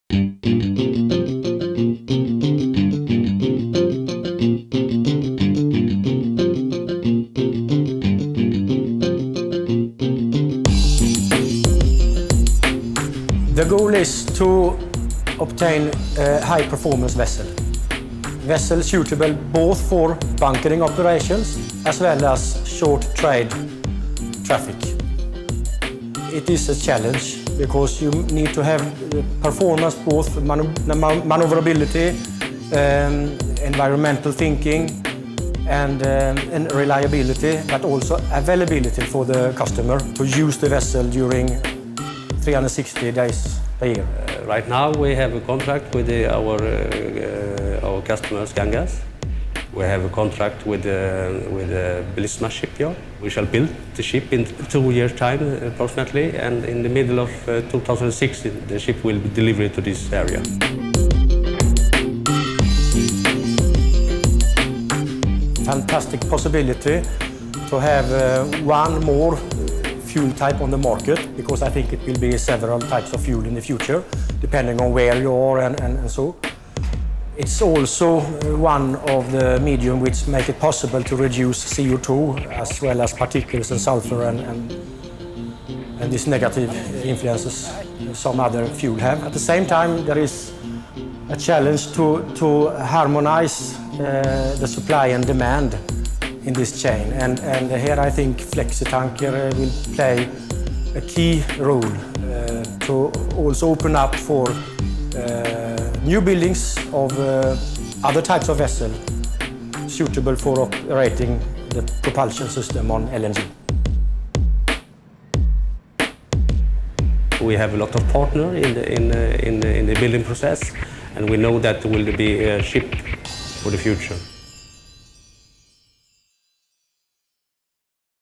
Flexi – a part of Pilot LNG
JIP Flexi is participating in the establishment of an LNG infrastructure via Pilot LNG by building a vessel with a fast, efficient and safe bunkering system for LNG bunkering in- and offshore, as well as developing efficient procedures for LNG bunkering.